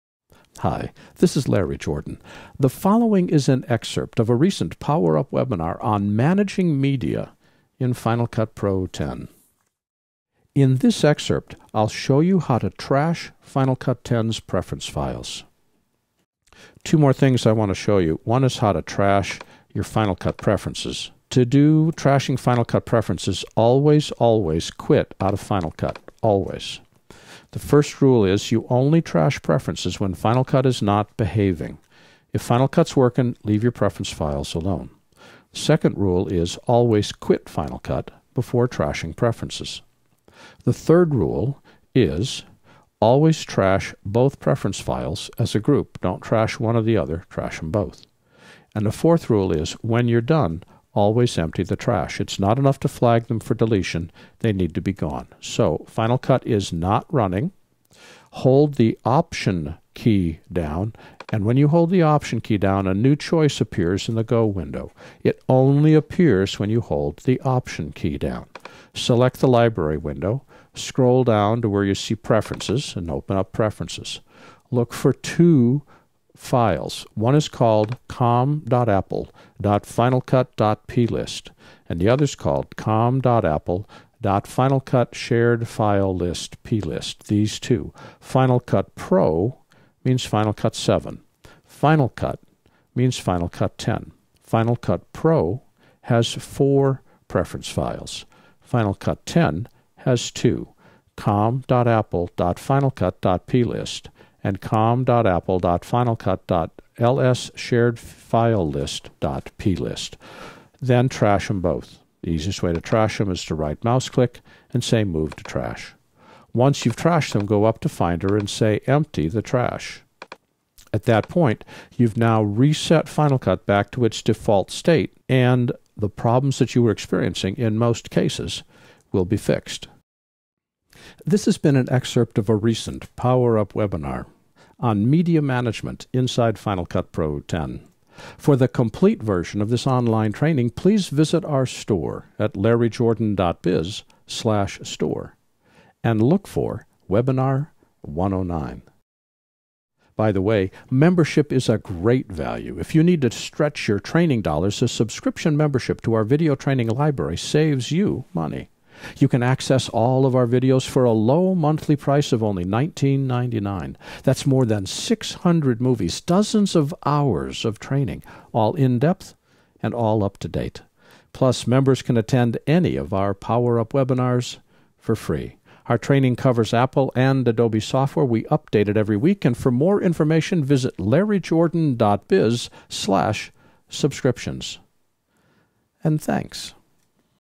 Trashing Preference Files in FCPX
- Wondering what to do when your Final Cut Pro X project is acting up?  Well, before shouting "Arrgh!" and throwing your hands in the air - two very helpful steps in troubleshooting - , the first thing you should do is try to trash your preference files in FCPX. Take a look at this excerpt from our recent "Managing Media in Final Cut Pro X" webinar, and let us quickly walk you through the process! - Enjoy!